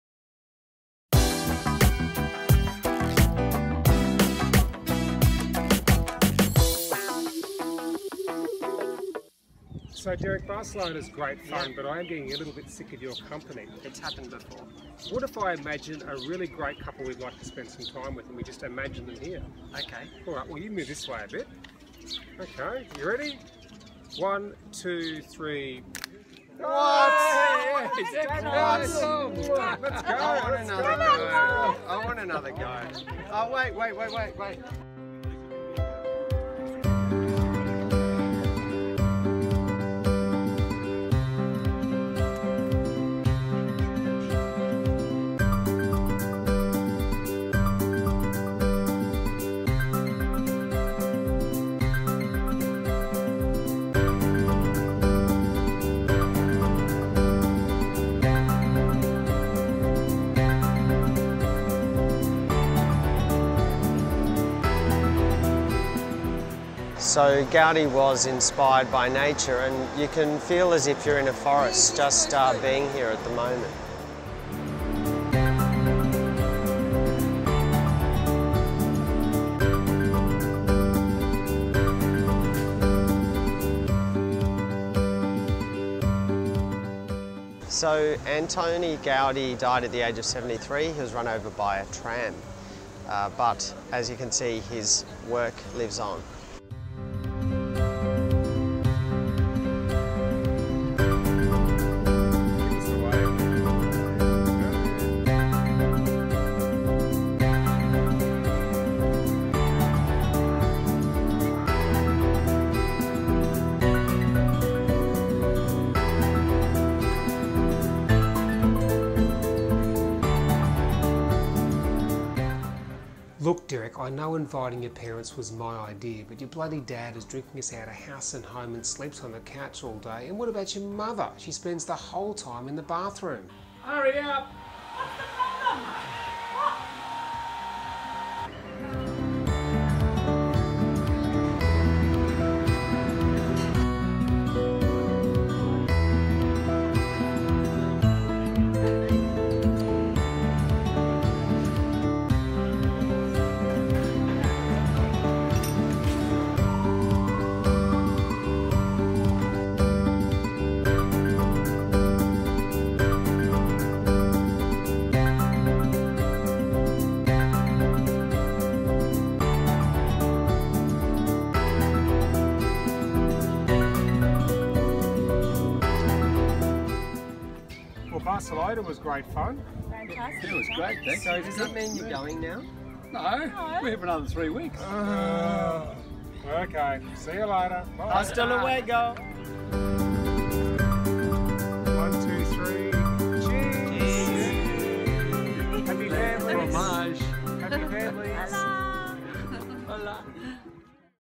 Tom & Derek Do Barcelona
Derek's parents arrive in Barcelona and explore the Spanish city